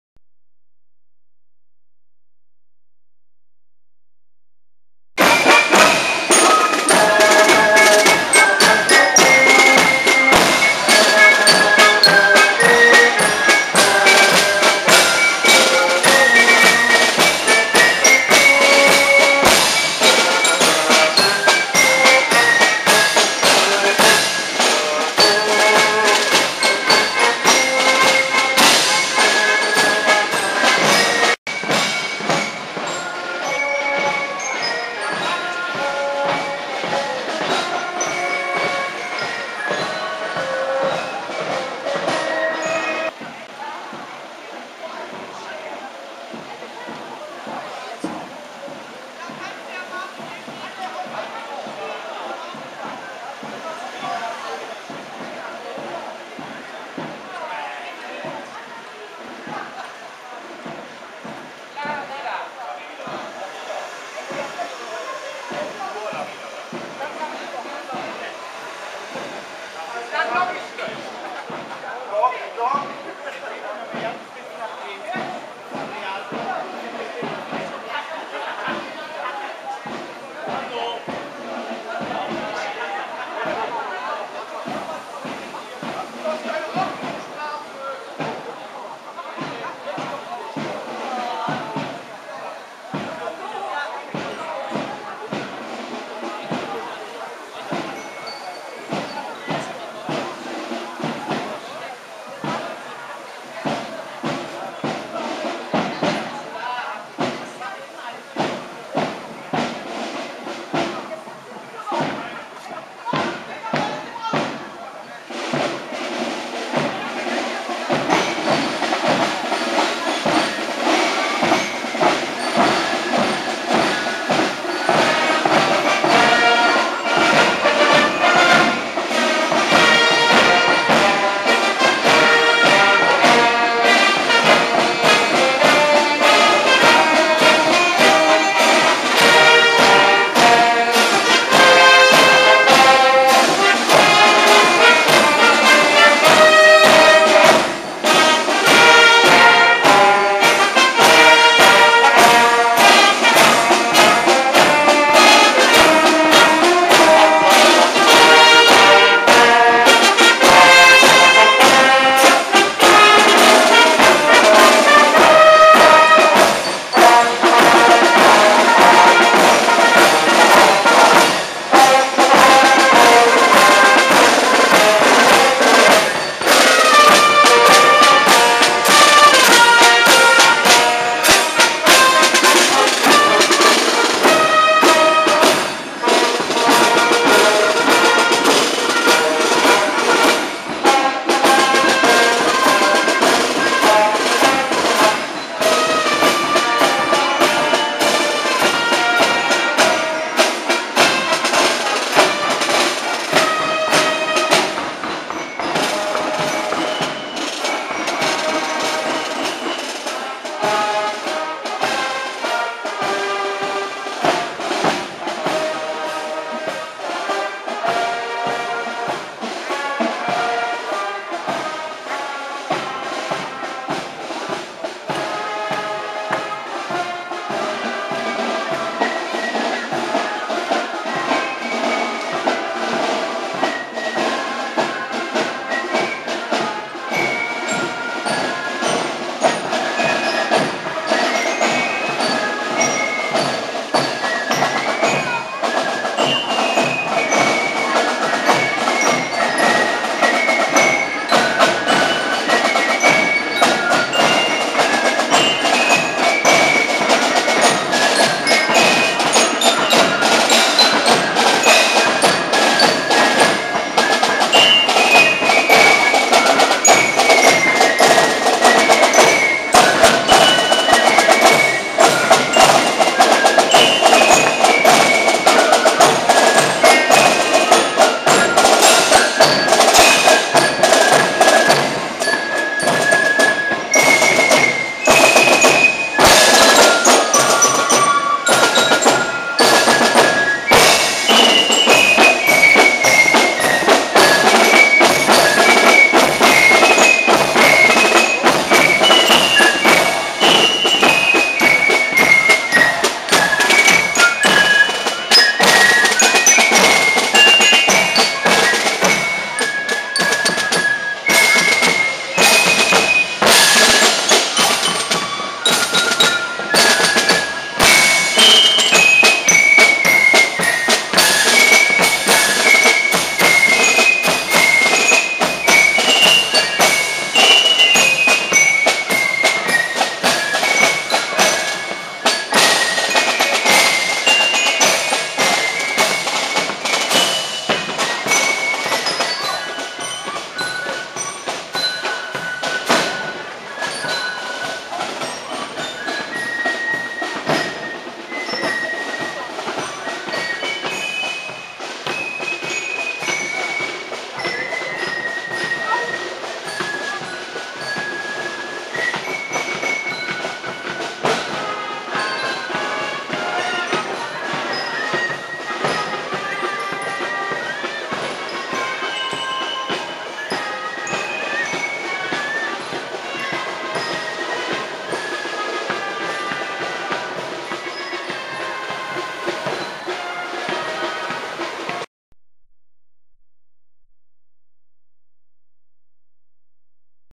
Schützenumzug Köln Humboldt
Schützenumzug in Köln Humboldt am 06.Juli 2012 von unserem Wohnungsfenster aus gesehen!